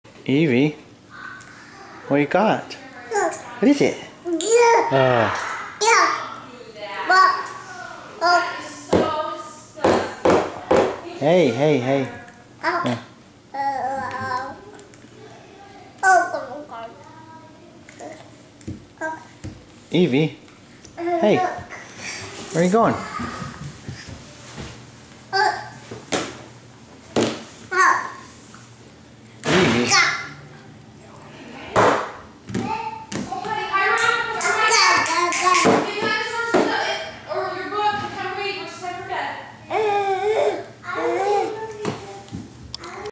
Evie playing with dad's phone
What else is there to do sitting on the old man's desk on a sunny Sunday afternoon?